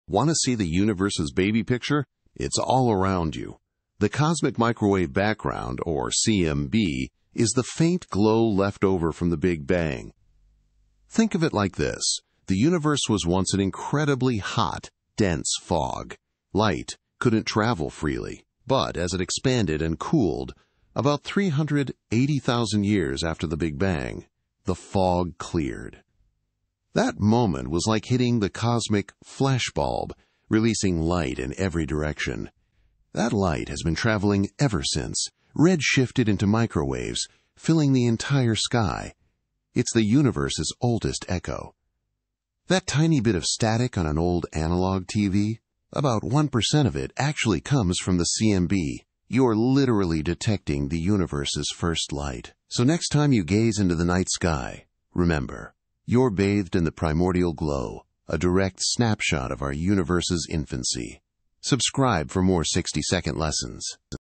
Universe's Baby Picture
Want to see the universe's baby picture? It's all around you! The Cosmic Microwave Background, or CMB, is the faint glow left over from the Big Bang.